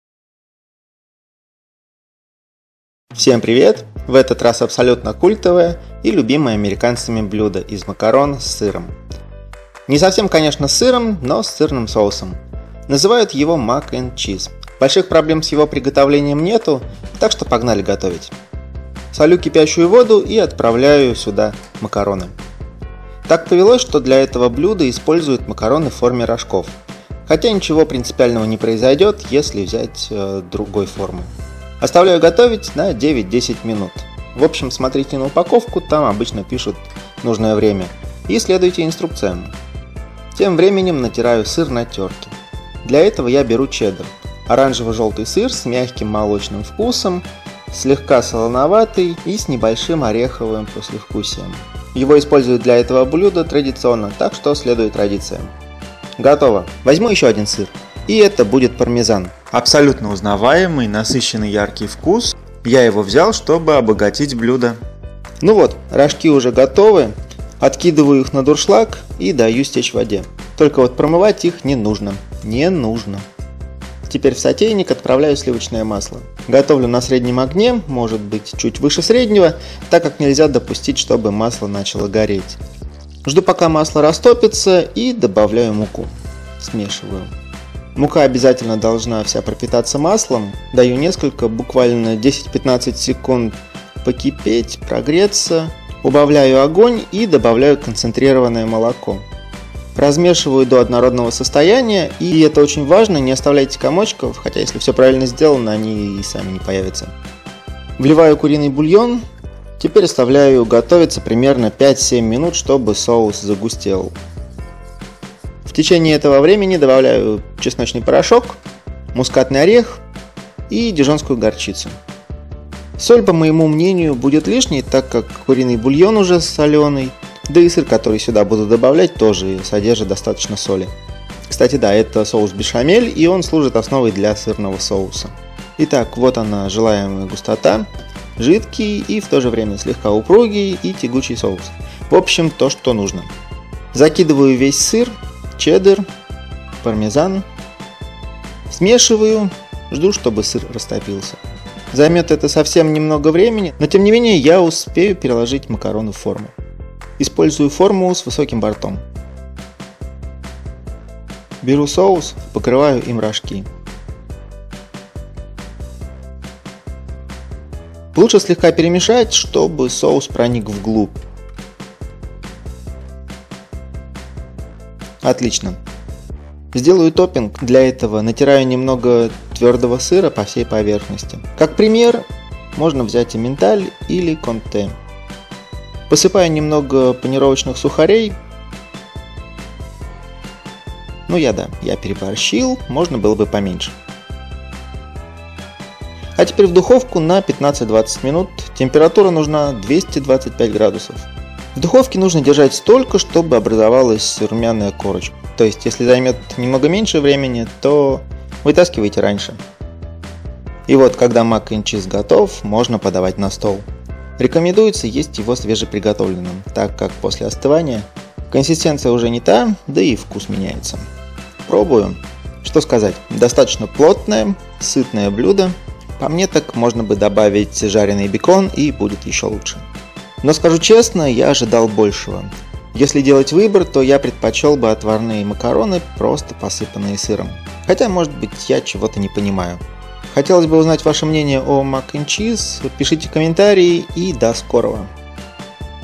Мак энд чиз / Mac&Cheese
Привет! Простое в приготовлении, очень сытное, культовое американское блюдо Мак энд чиз.

Что нужно:
250 гр. макарон (рожки)
250 гр. сыра чеддер
50 гр. сыра пармезан
330 мл. концентрированного молока
250 гр. куриного бульона
1 ч.л. порошка чеснока
1 ч.л. мускатного ореха
1 ч.л. дижонской горчицы
40 гр. сливочного масла
40 гр. муки
20 гр. сыра комте или эмменталь
15-20 гр. панировочных сухарей

Рецепты Bon Appétit
канал бон аппетит
Бон аппетит